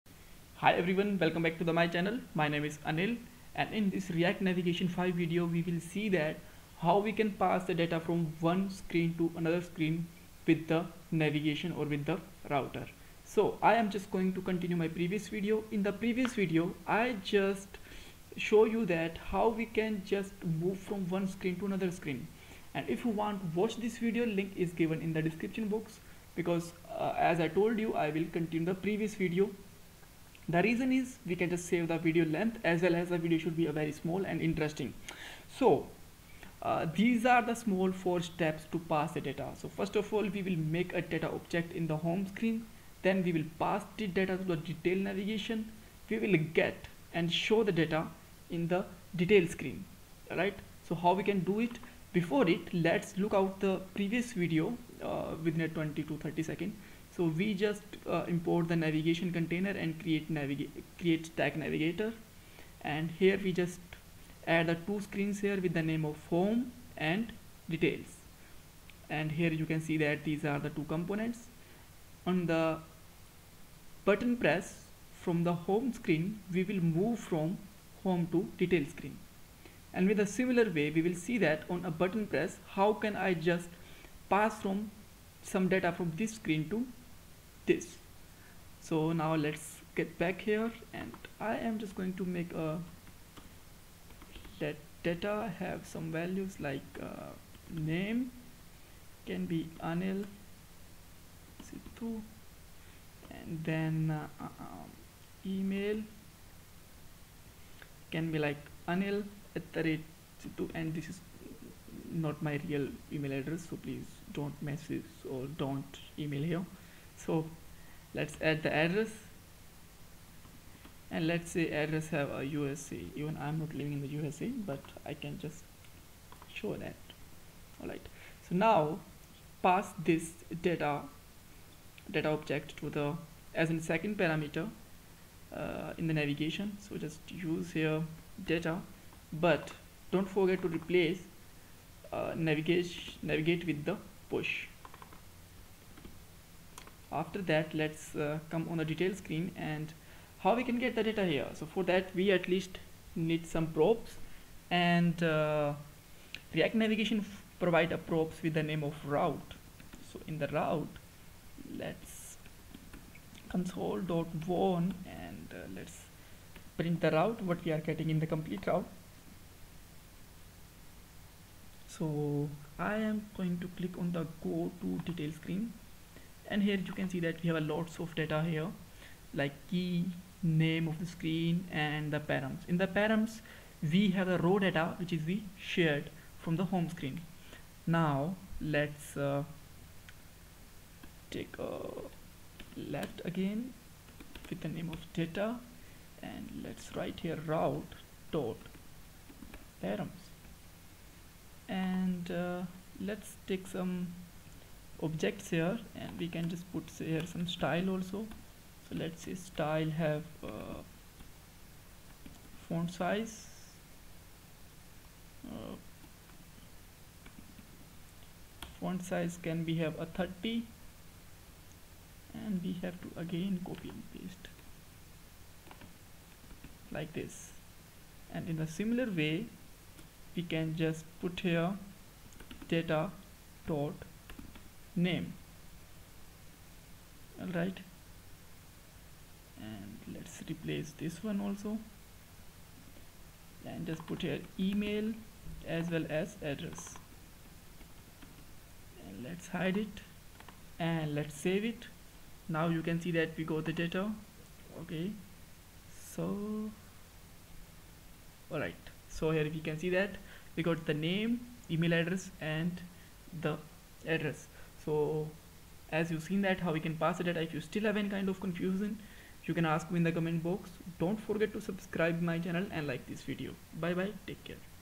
React Navigation 5 tutorial #3 pass data between screens
in this react-navigation English tutorial, we learn how to use its version 5 with react native and how to pass data from one screen to another screen stack in navigation it from scratch. this video is made by anil Sidhu in the English language